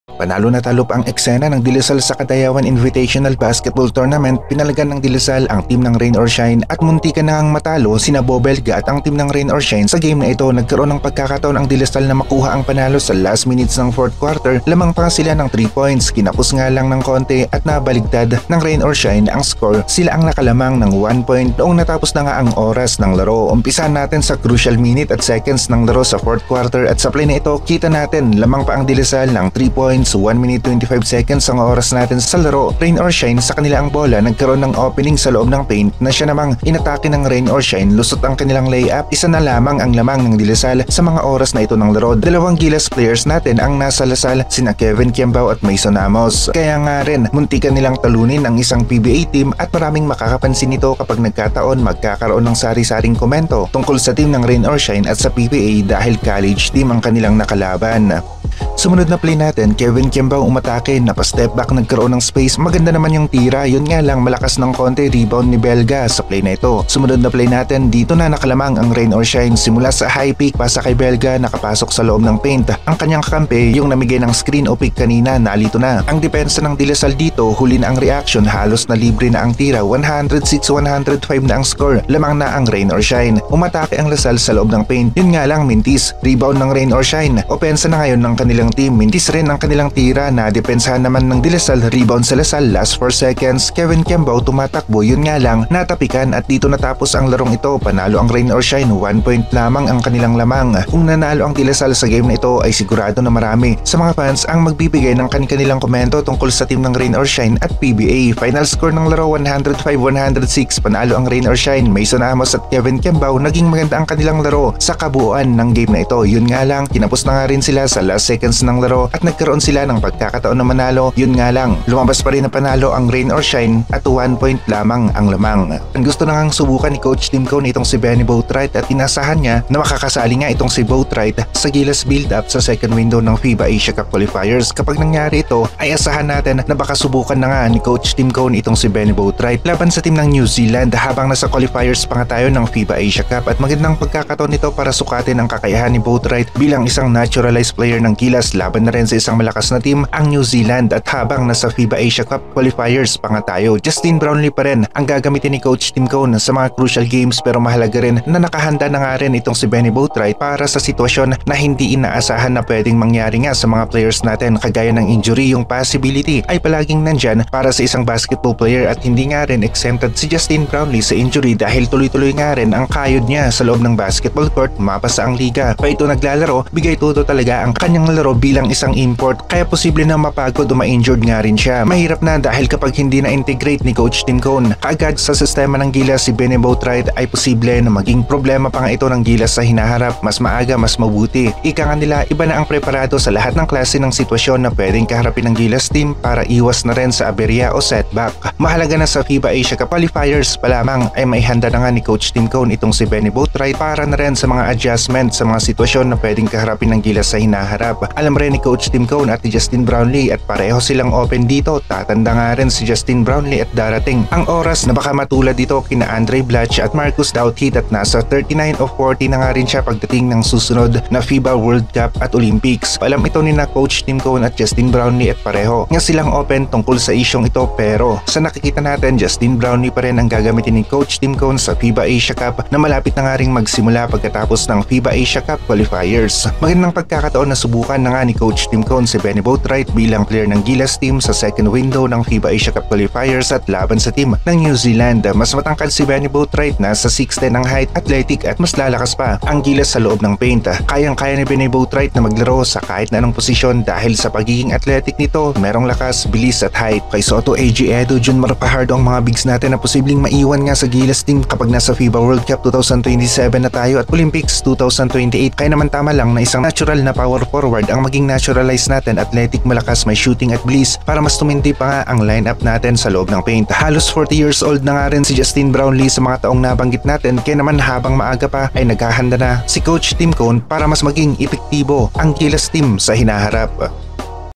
Nagkasubukan KQ VS Belga. Kai Sotto Edu Fajardo Boatwright Rotation lakas sa ilalim. Pro VS Non Pro.
Kai Sotto AJ Edu June Mar Fajardo Boatwright Rotation.
Rain or shine PBA pro team VS College Non Pro Team.
Kevin Quiambao VS Beau Belga. Napa No No si Belga.